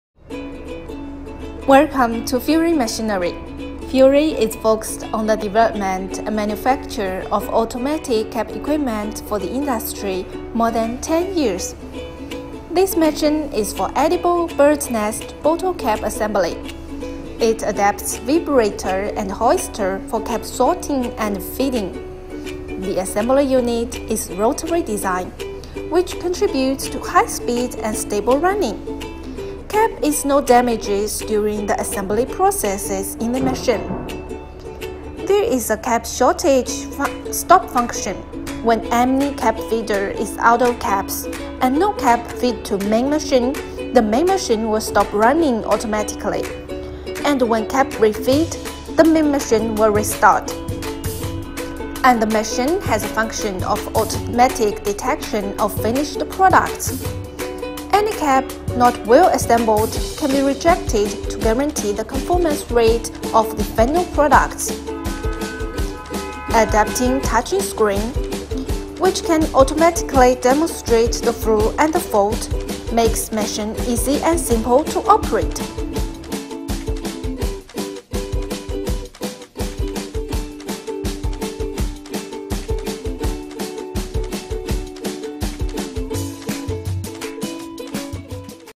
Edible Bird's Nest Bottle Cap Assembly Machine
Furi cap assembly machine for edible bird's nest bottle cap assembly. It adopts vibrator and hoister for cap sorting and feeding. The assembly unit is rotary design, which contributes to high speed and stable running. Cap is no damages during the assembly processes in the machine.